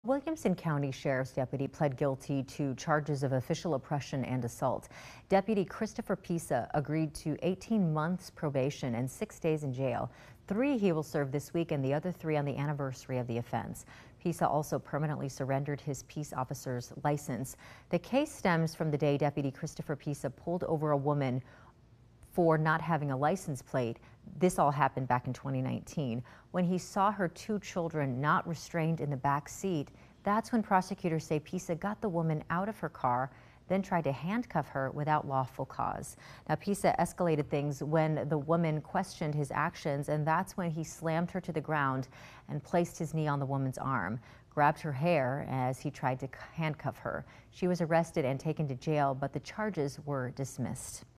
WCSO deputy pleads guilty in 2019 excessive force case, gets 18 months probation
A Williamson County Sheriff’s deputy entered a guilty plea on charges of official oppression and assault in connection with an excessive force case from 2019, according to a news release from District Attorney Shawn Dick’s office.